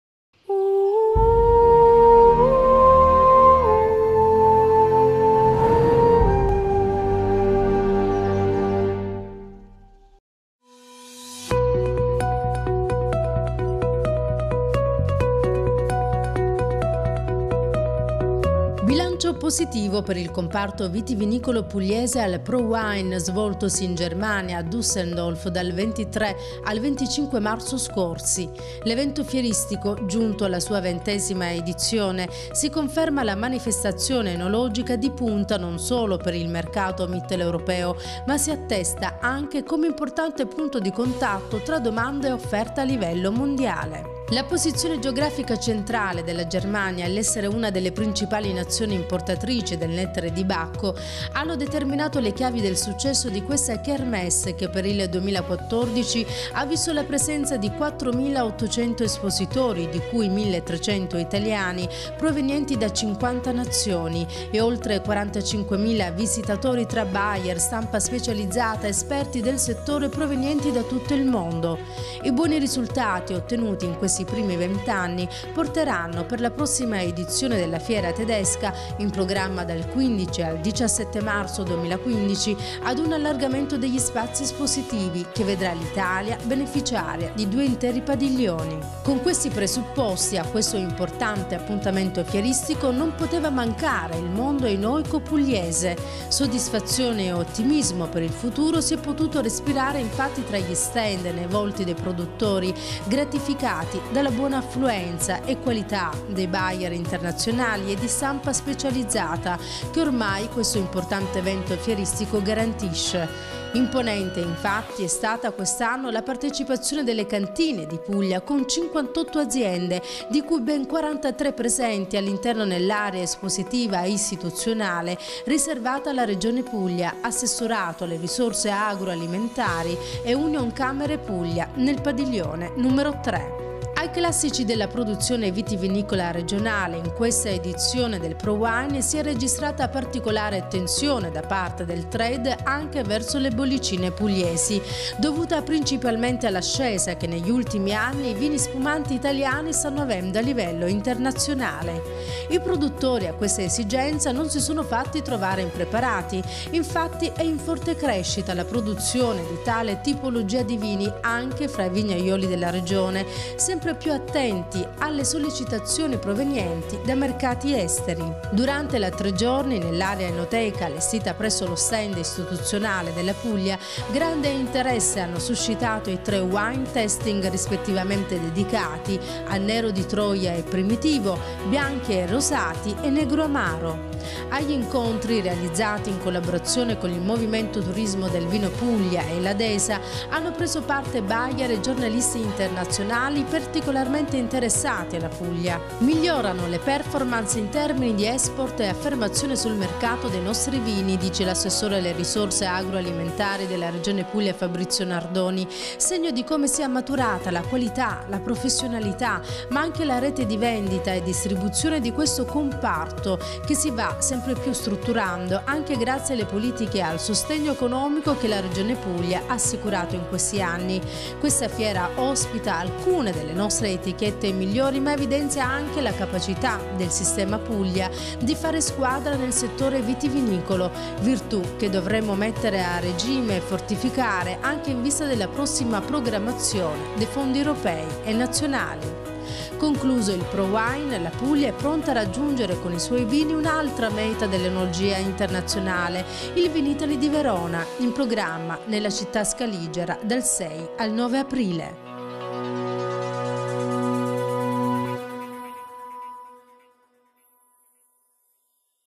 Speciale ProWein 2014. Cronaca del nuovo successo del vino di Puglia a Dusseldorf
4.800 espositori (di cui 1.300 italiani) provenienti da 50 nazioni e oltre 45.000 visitatori tra buyer, stampa specializzata, esperti del settore provenienti da tutto il mondo. ProWein, a Dusseldorf in Germania, giunto alla sua ventesima edizione, si conferma la manifestazione enologica di punta non solo per il mercato mitteleuropeo ma si attesta anche come importante punto di contatto tra domanda e offerta a livello mondiale. Importante è stata la partecipazione delle cantine di Puglia con 58 aziende, di cui ben 43 presenti all'interno nell'area espositiva istituzionale riservata alla Regione Puglia Assessorato alle Risorse Agroalimentari e Unioncamere Puglia nel Padiglione 3.Durante la tre giorni, nell'area "enoteca", allestita presso lo stand istituzionale della Puglia, grande interesse hanno suscitato i tre wine tasting rispettivamente dedicati a Nero di Troia e Primitivo, Bianchi e Rosati e Negroamaro. Agli incontri, realizzati in collaborazione con il Movimento Turismo del Vino Puglia e la DE.S.A. (Deutschland Sommelier Association), hanno preso parte buyer e giornalisti internazionali particolarmente interessati alla Puglia. Concluso il Prowein, la Puglia è pronta a raggiungere con i suoi vini un'altra meta dell'enologia internazionale: il Vinitaly di Verona, in programma nella città scaligera dal 6 al 9 aprile.